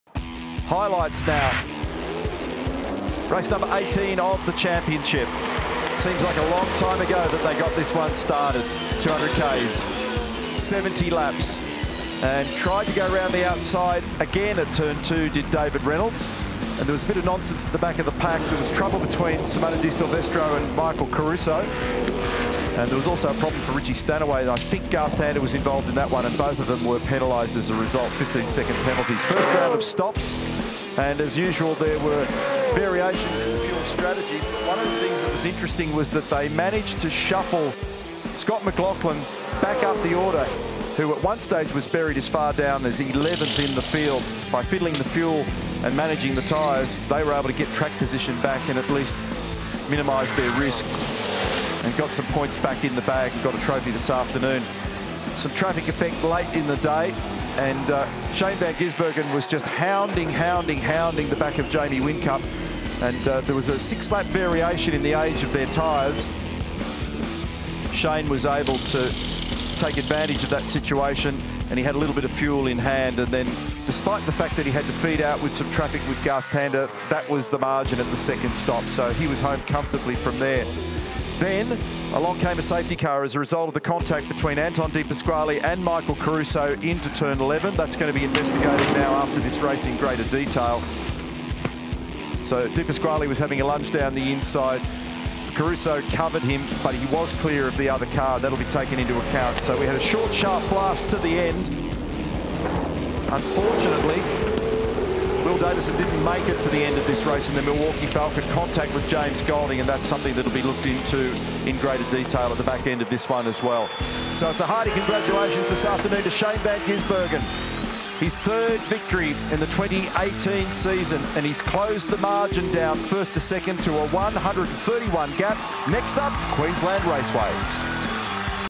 Highlights: Race 18 2018 Watpac Townsville 400
Shane van Gisbergen beats Red Bull Holden Racing Team-mate Jamie Whincup to take out the second leg of the 2018 Watpac Townsville 400.